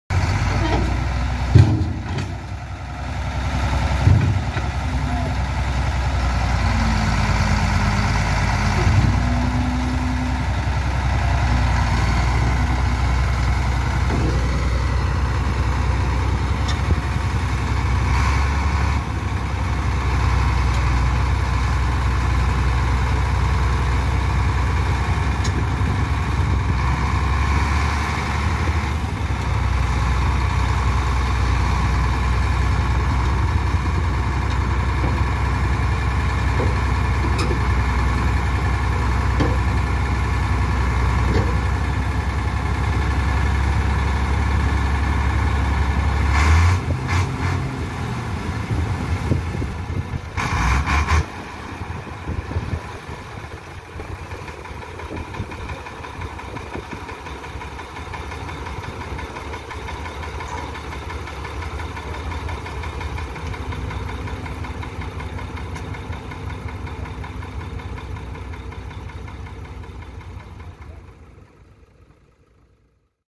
Komatsu D70 Bulldozer Loading On Lowbed Trailer
Alternative way to load dozer on lowbed with no loading ramp.